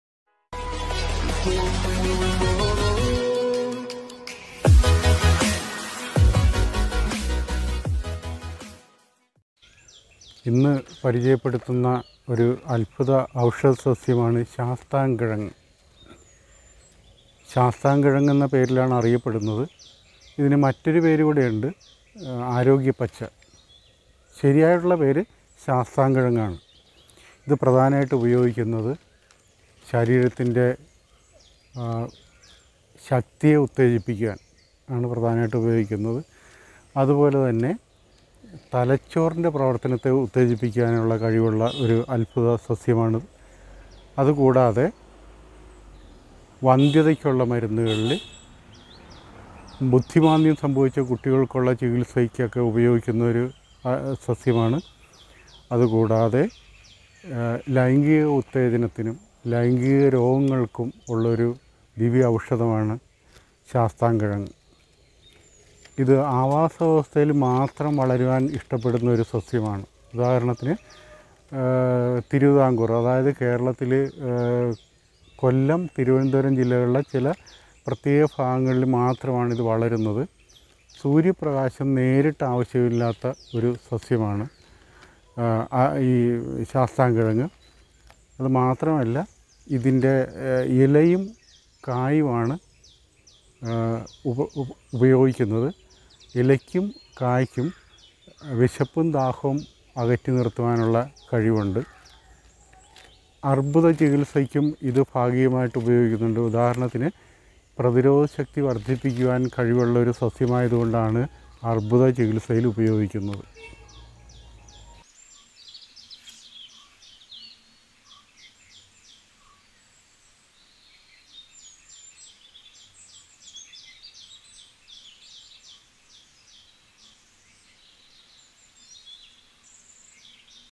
ആരോഗ്യപ്പച്ച | Arogyapacha malayalam | Trichopus zeylanicus | ചാത്തൻകിഴങ്ങ് | ജീവനി
What happened to Trichopus zeylanicus (Arogyapacha)?
Medicinal Importance of Arogyapacha
Arogyapacha project

ബുദ്ധിമാന്ദ്യത്തിന് ശാസ്താം കിഴങ്ങ്

Arogyapacha - It has been in use for centuries by the Kaani tribal community of the Agastya Koodam ranges in Kerala, India, for its medicinal properties.

Modern Indian scientists learned the medical properties of TrichopusZeylanicus in December 1987 while on a scientific expedition to the Agasthia Hills in the Western Ghats. They noticed that their guides, belonging to the Kaani tribe, were very energetic in sharp contrast to themselves. They had walked for several hours with the scientists, but the difference was that they ate the fruits of a wild plant T. zeylanicus as they walked. It was found from the Kaani men that it was indeed the fruits they were eating that made them energetic, a fact about the plant well known to the tribe for ages. Historically, the members of the tribes called the plant 'chathan kalanji' literally meaning 'satan destroyer'. The more popular name of the plant, "Arogya pacha" (literally meaning "the green that gives strength") was given to it by the scientists.